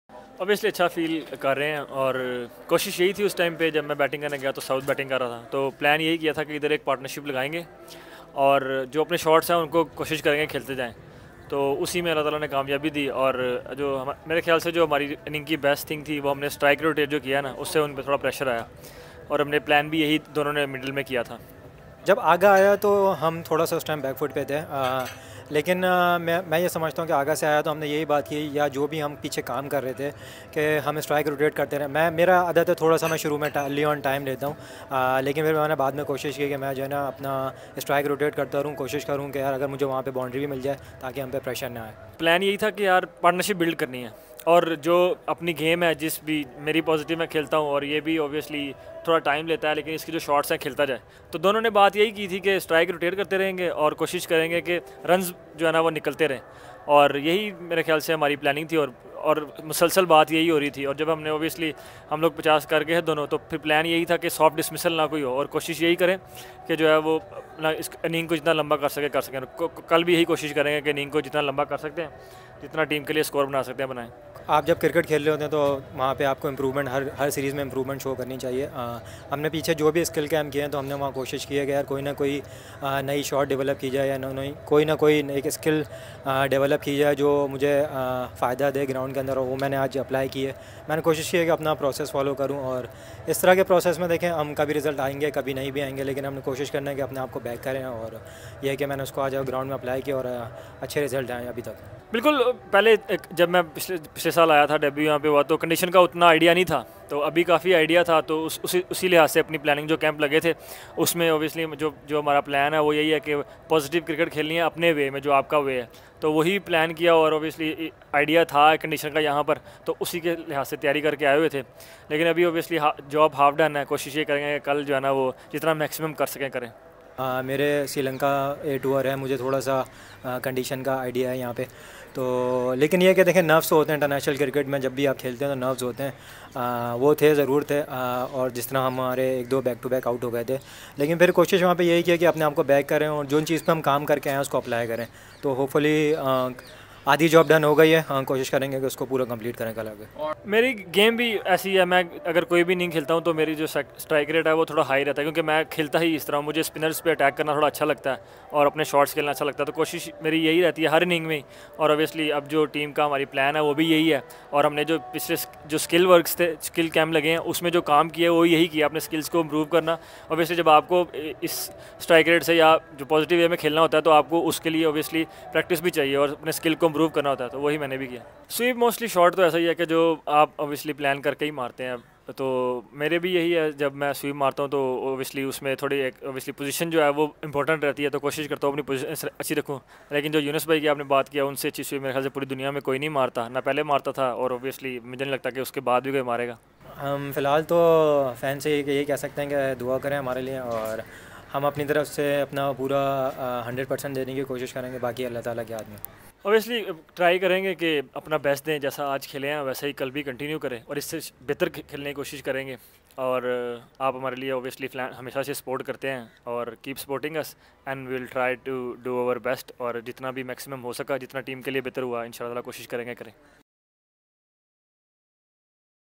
Saud Shakeel & Salman Ali Agha share insights from their brilliant partnership in Galle | PCB | MA2L
Saud Shakeel & Salman Ali Agha share insights from their brilliant partnership in Galle